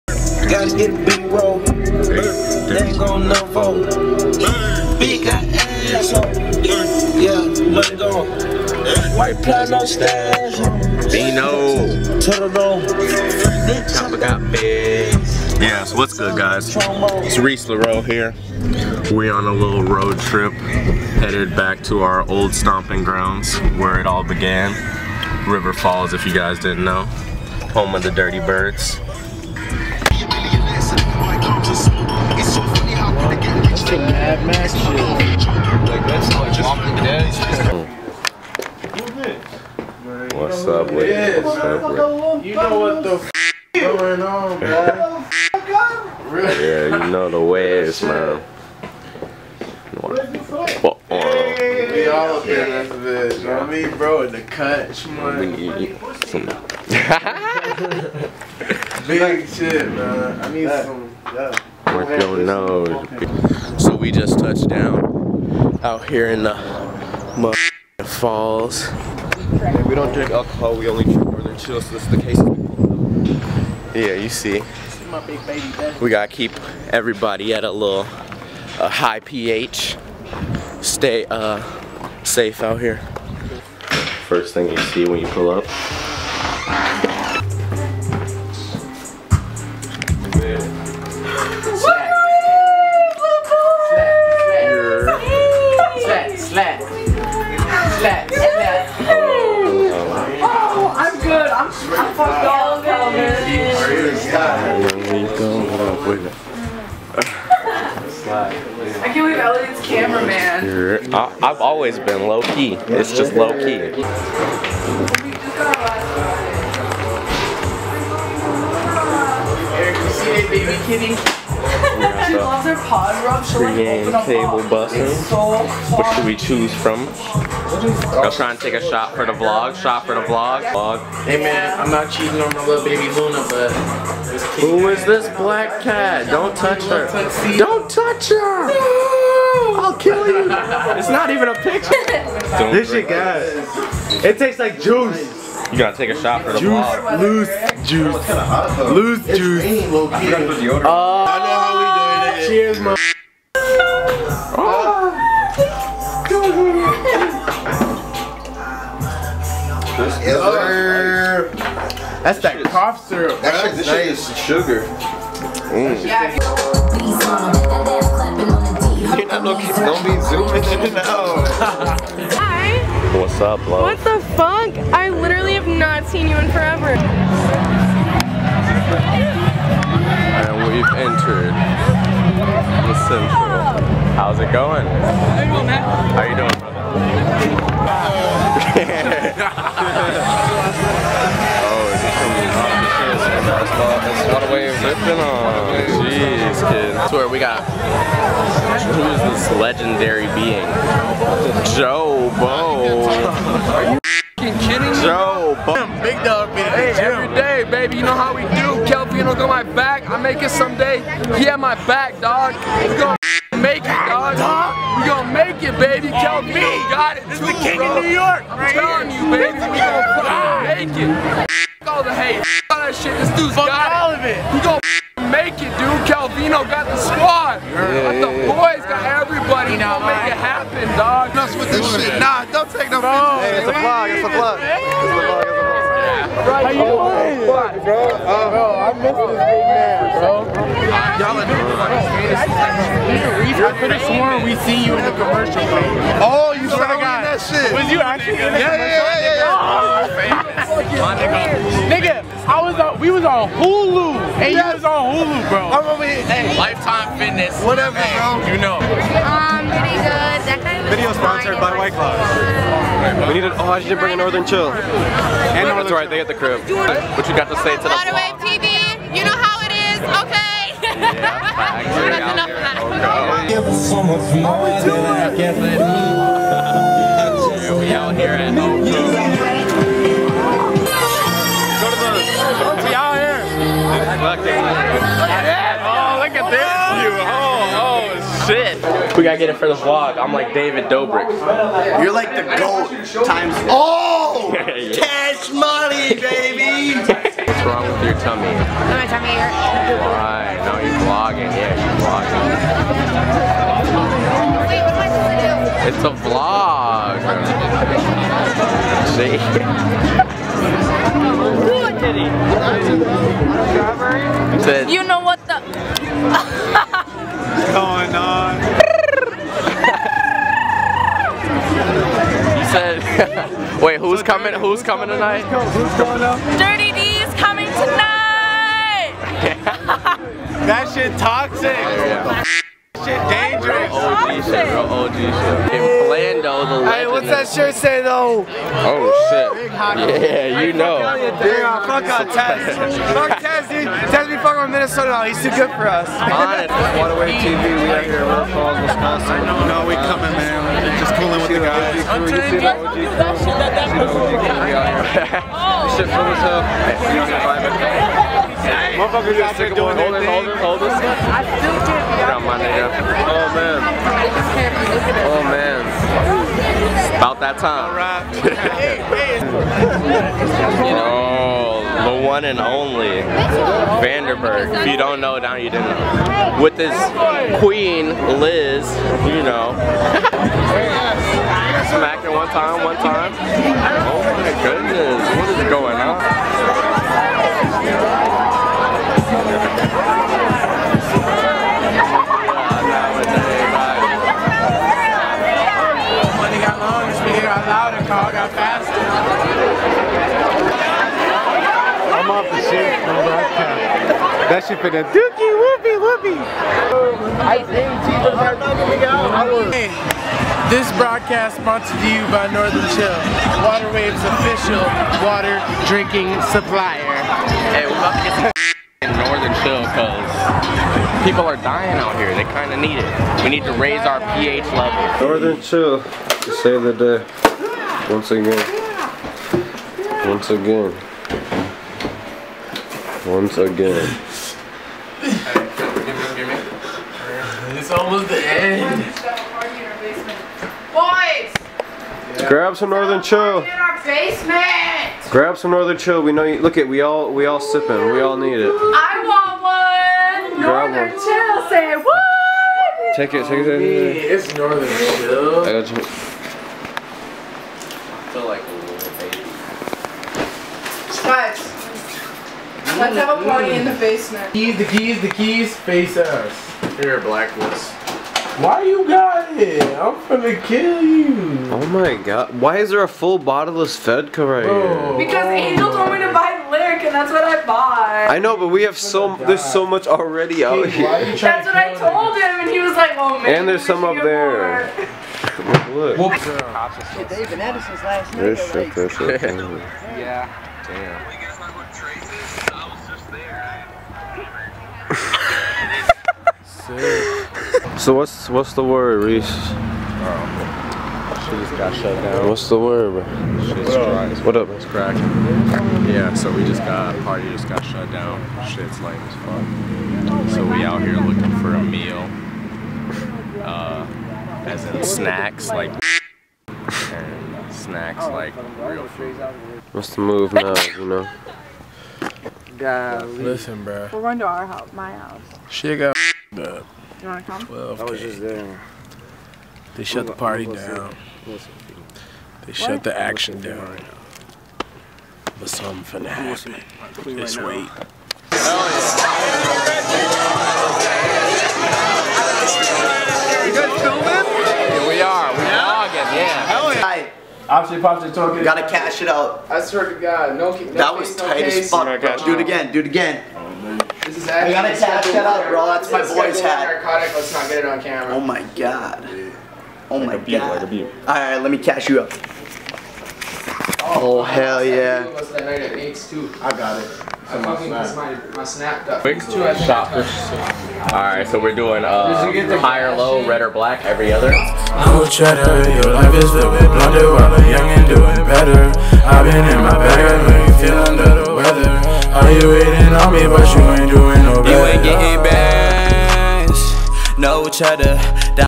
UW River Falls Homecoming 2019 VLOG - Water Wave TV
We took a trip back to our old stomping grounds of River Falls WI. and met up with some of our good friends and had a good time!